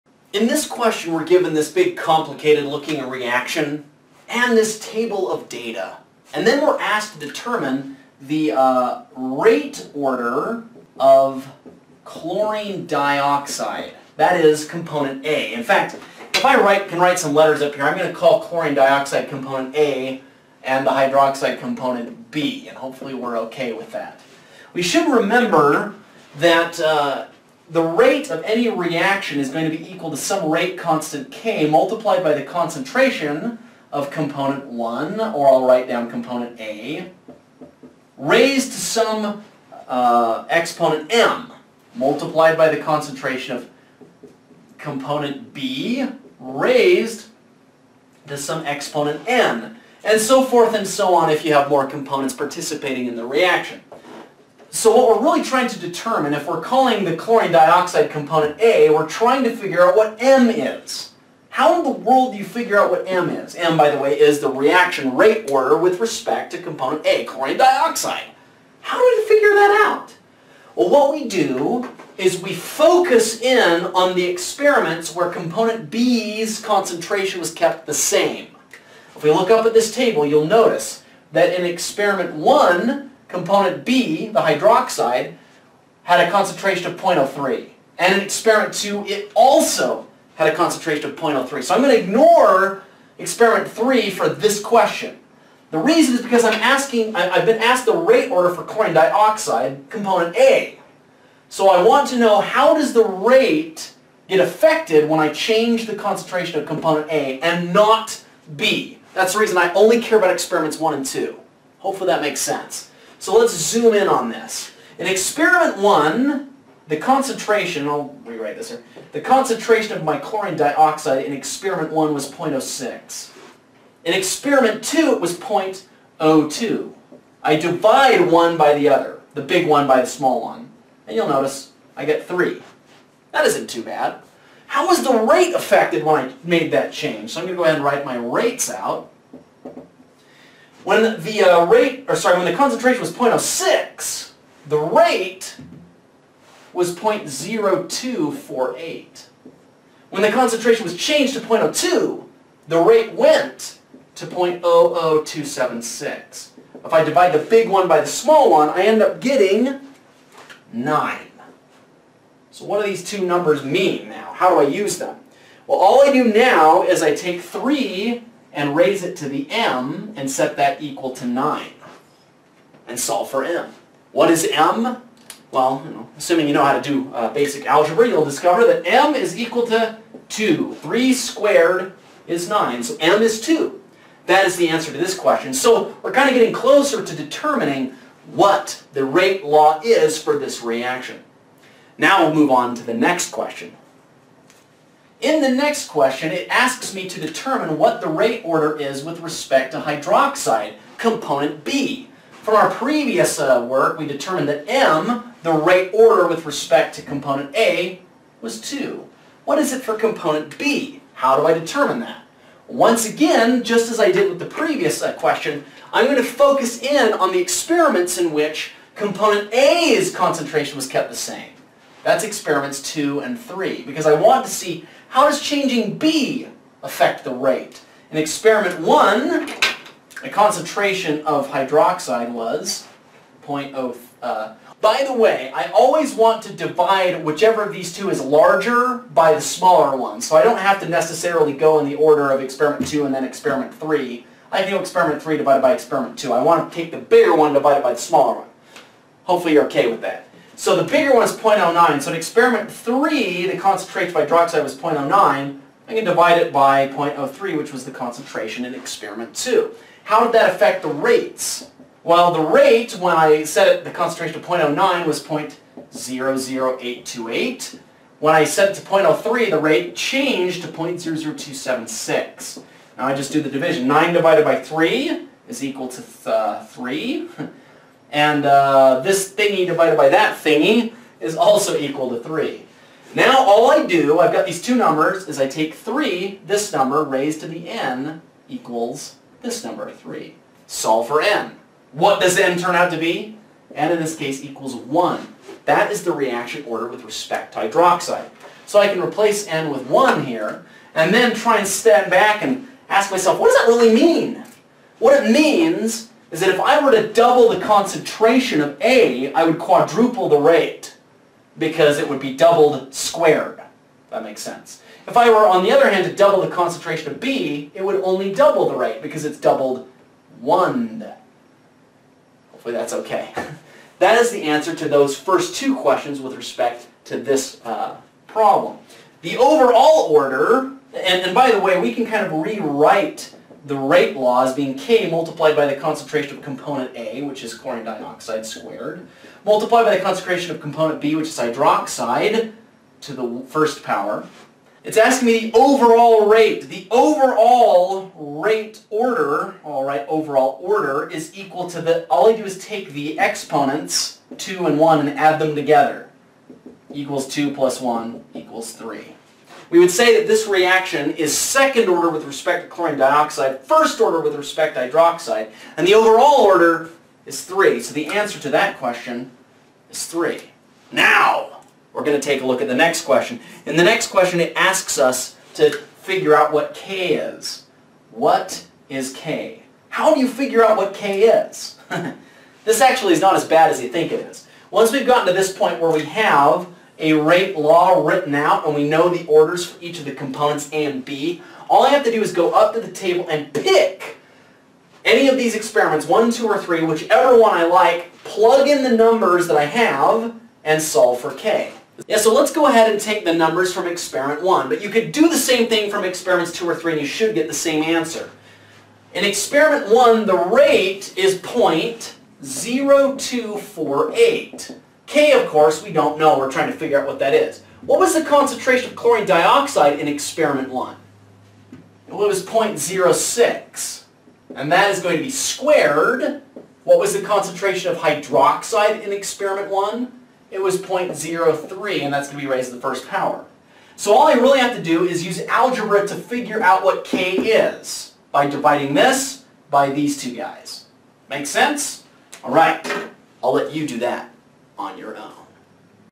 Chapter 14 – Chemical Kinetics: Part 10 of 17
In this video I do an example problem that involves determining a reaction’s rate law, rate law constant, and reaction orders from experimental data.